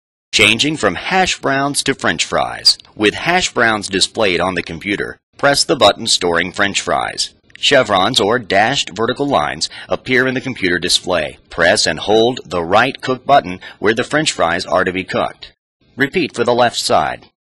008 Hash Browns to French Fries
Switch from Hash Browns to French Fries on a Frymaster LOV fryer for McDonald's